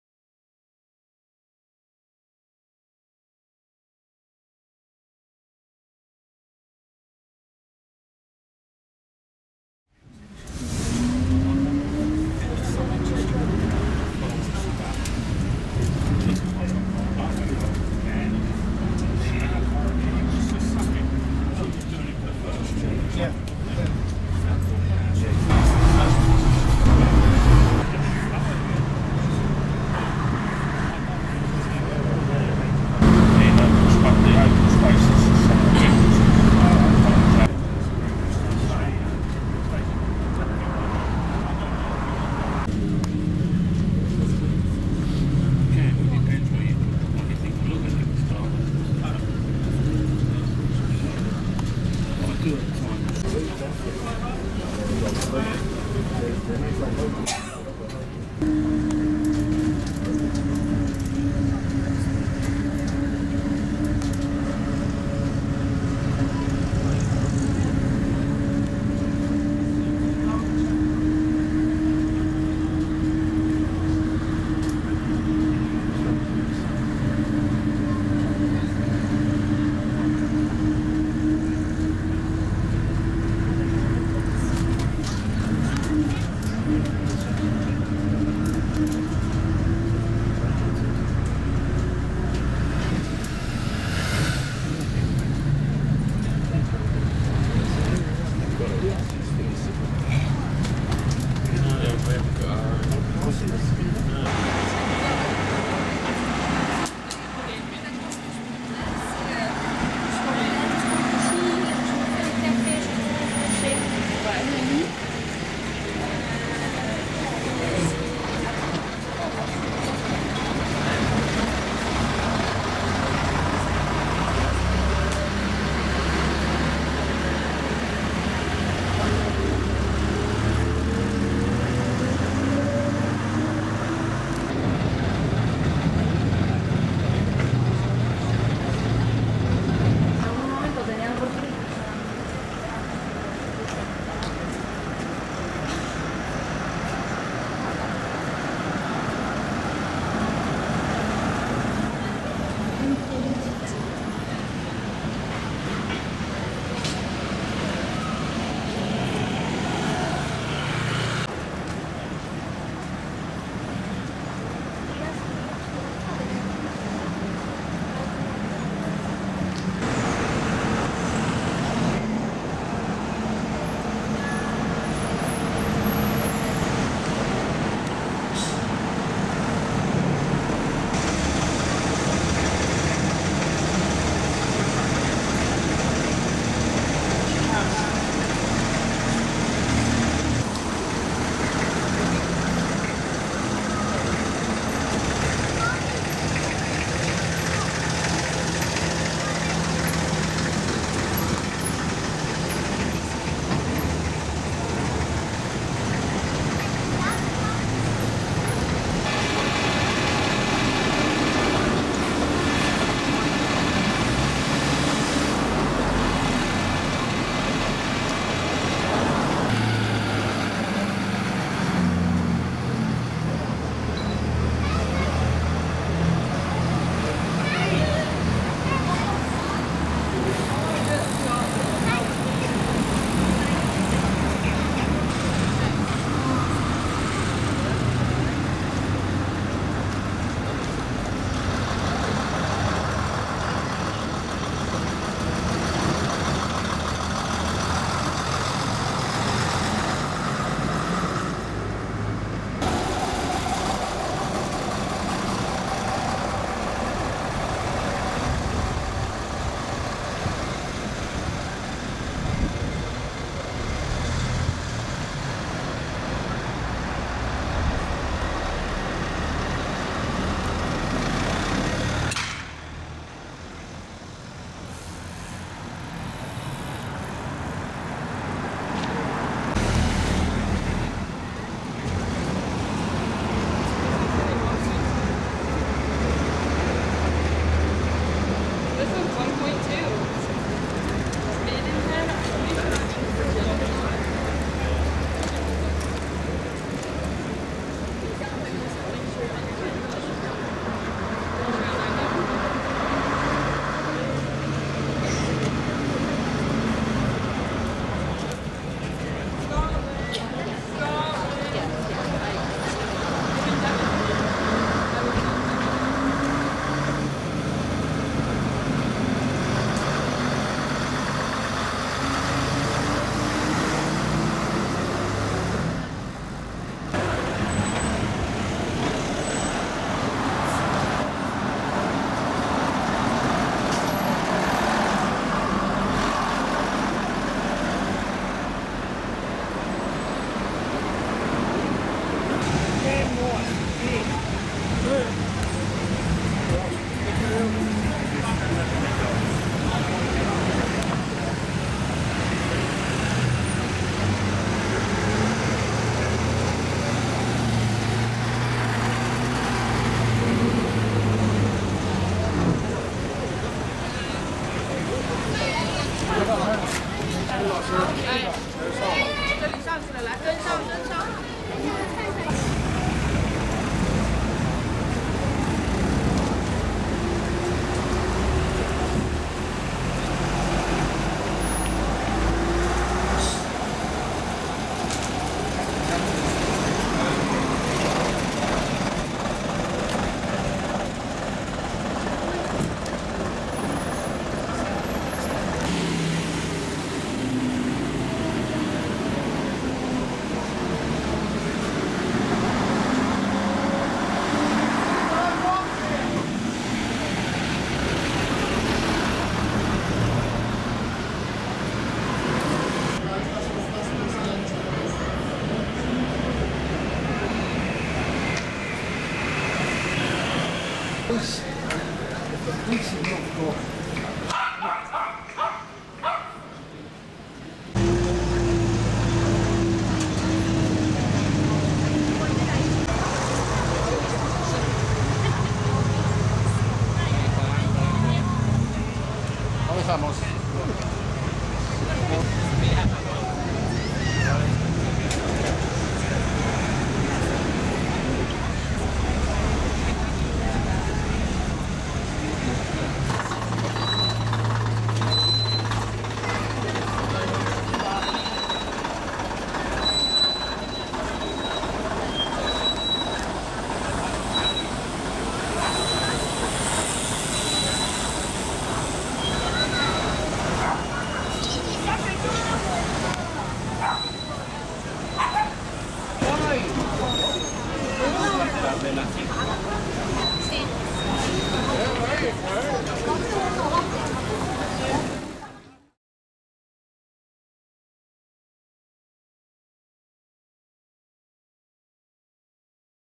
Windsor Classic bus running day 2024 1
Amersham & District Motorbus Society have once again presented an excellent day of vintage bus running, made even better by the iconic location - Windsor town and castle - and the glorious sunshine. Of course on this beautiful summer Sunday the town was full of tourists, locals and - today - mostly elderly bus spotters! If you weren't able to make it, this 8 minute video should give you a flavour of the event.